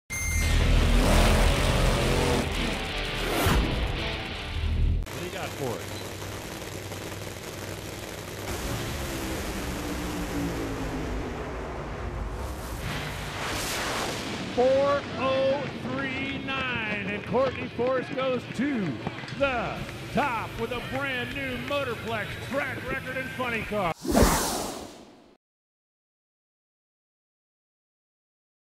Courtney Force sets new track record in Dallas | NHRA
Courtney Force raced to the Funny Car qualifying lead Friday at the AAA Texas NHRA FallNationals with a track record performance at Texas Motorplex. Force set both ends of the track record in her Traxxas Ford Mustang with a 4.039-second pass at 314.90 mph.